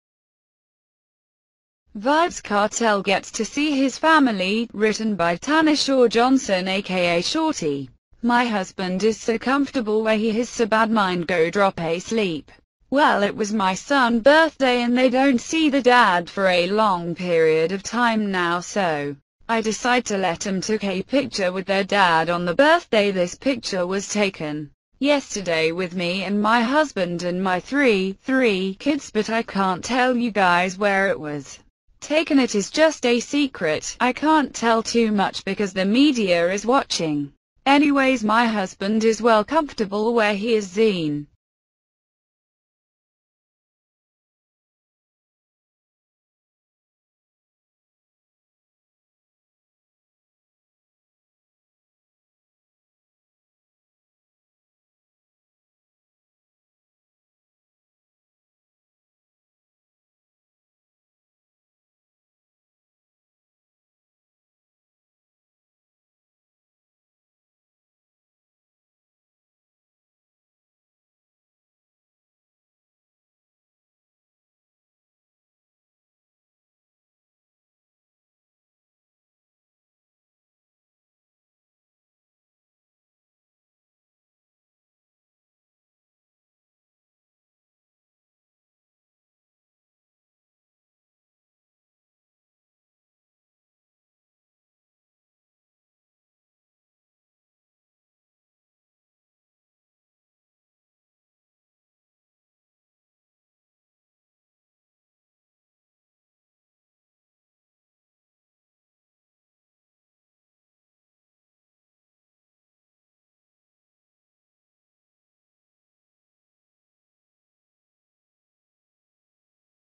(Photo) Kartel out for Christmas!
Vybz Kartel Gets To See His Family. Written By: (Tanesha Johnson Aka Shorty) My Husband Is So Comfortable Where He His So Badmind Go Drop A Sleep ! Well It Was My Son Birthday & They Don’t See Their Dad For A Long Period Of Time Now SO I Decide To Let Them Took A Picture With Their Dad On Their Birthday This Picture Was Taken Yesterday With Me & My Husband & My Three (3) Kids But I Can’t Tell You Guys Where It Was Taken It Is Just A Secret , I Can’t Tell Too Much Because The Media Is Watching .